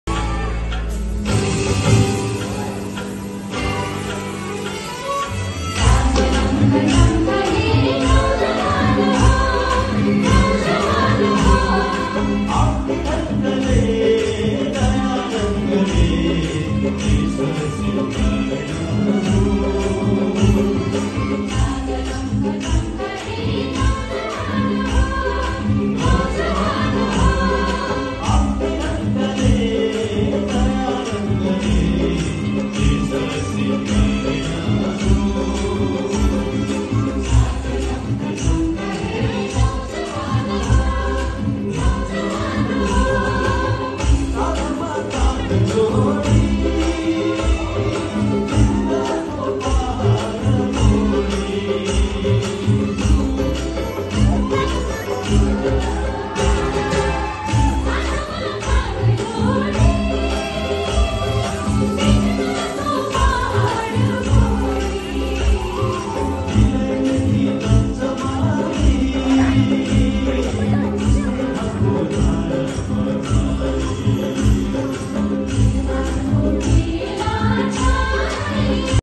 Nepali Folk music arkestra on the occasion of golden jublee of Ratriya nach ghar@Kathmandu 2079
Live presentation of Nepali Folk music arkestra on the occasion  of golden jublee of Ratriya Nachghar @Kathmandu 2079